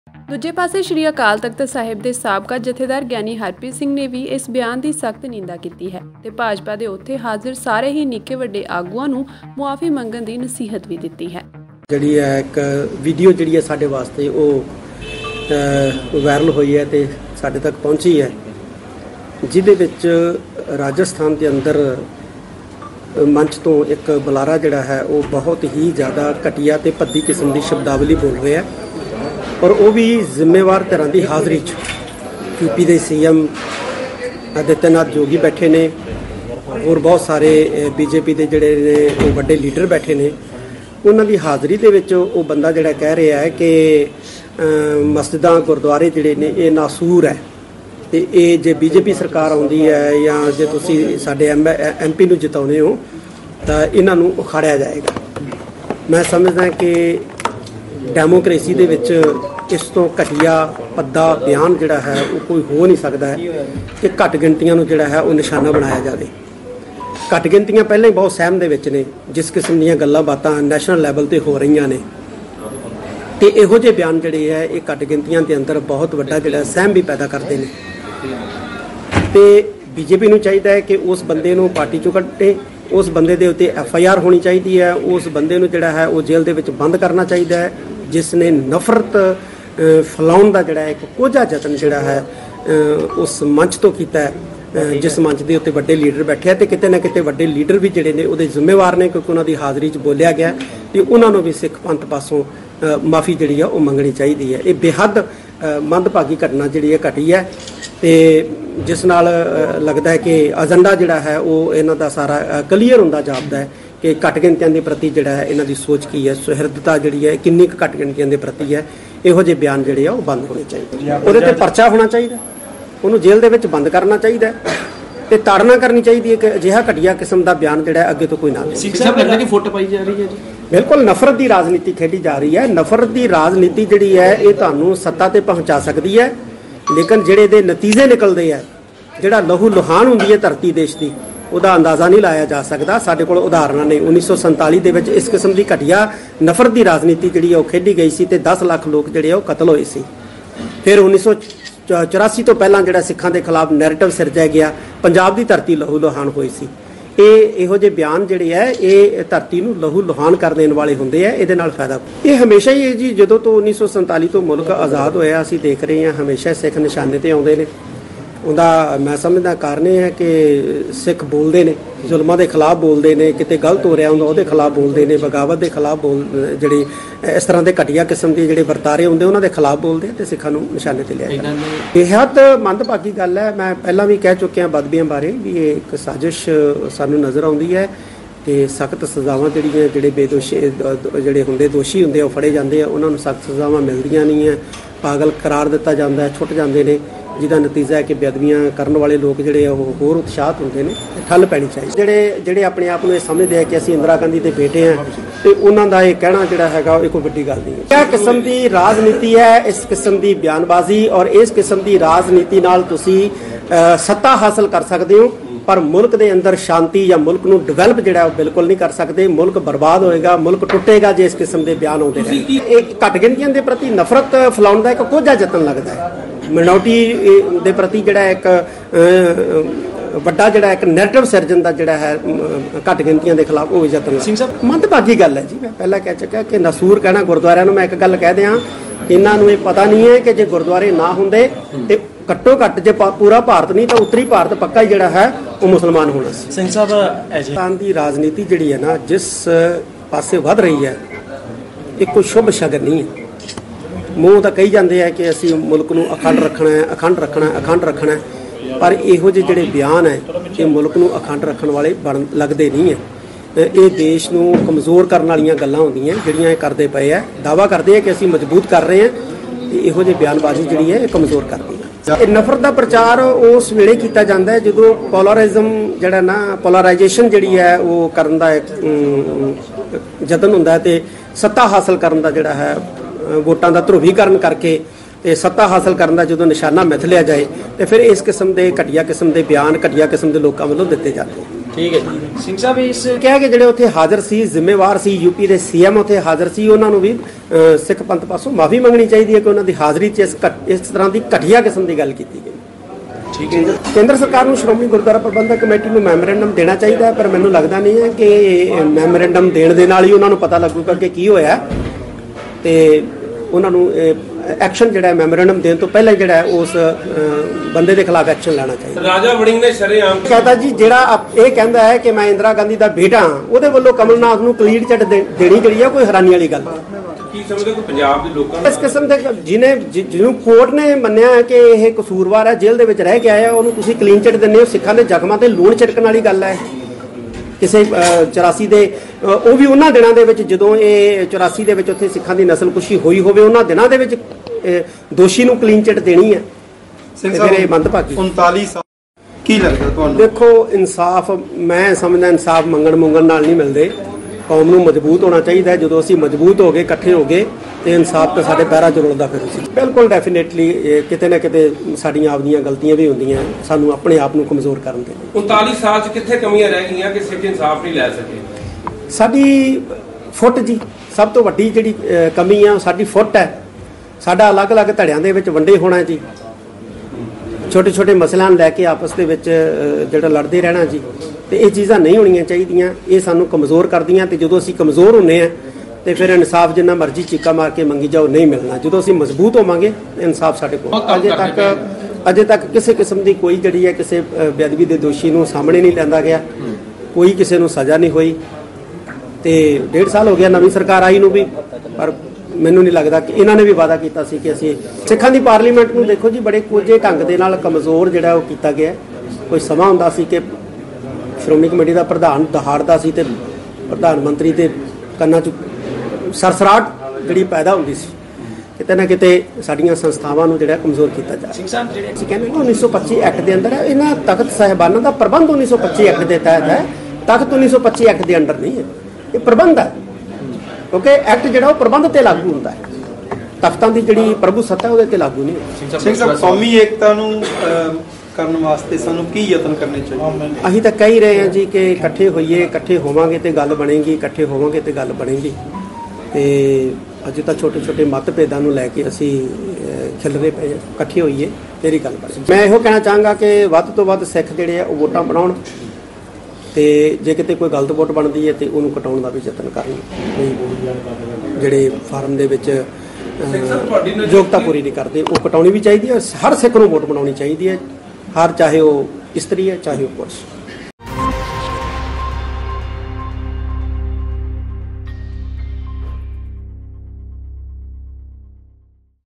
BJP ਆਗੂਆਂ ਵੱਲੋਂ ਰਾਜਸਥਾਨ ਚ ਗੁਰਦੂਆਰਾ ਸਾਹਿਬਾਨਾਂ ਤੇ ਦਿੱਤੇ ਬਿਆਨ ਦੀ ਸਾਬਕਾ ਜੱਥੇਦਾਰ ਨੇ ਕੀਤੀ ਨਿੰਦਾ
TV84
36-01 37TH AVENUE